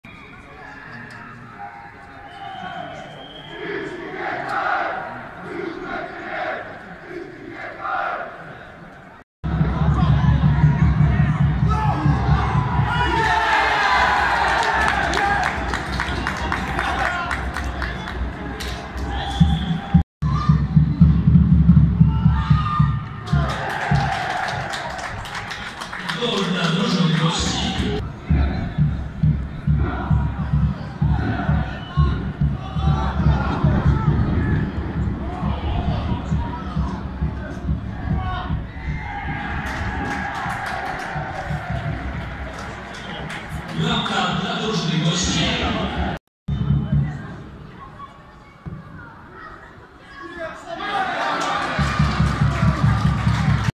Ultra Skrótem GARBARNIA - GKS'71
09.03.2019 skrót meczu Garbarnia Kraków - GKS '71 Tychy 1:3 Rozgrywek

Stadion Miejski Kraków. Obiekt zastepczy RKS Garbarnia Kraków.